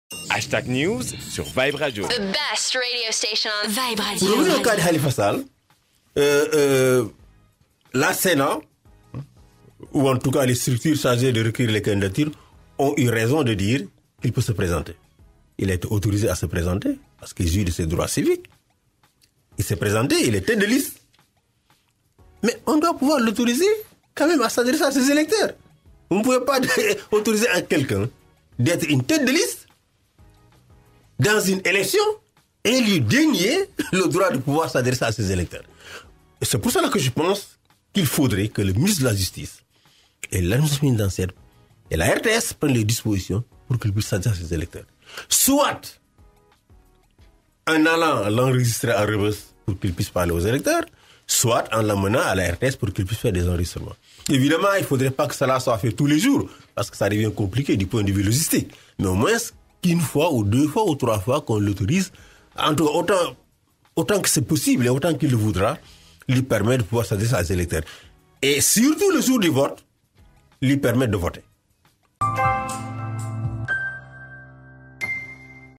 Seydi GASSAMA estime que Khalifa Sall à le droit de s'adresser à ses électeurs
Le patron d’Amnesty International Sénégal estime que  Khalifa Sall à le droit de s'adresser à ses électeurs  et que les autorités devraient mettre les moyens pour cela.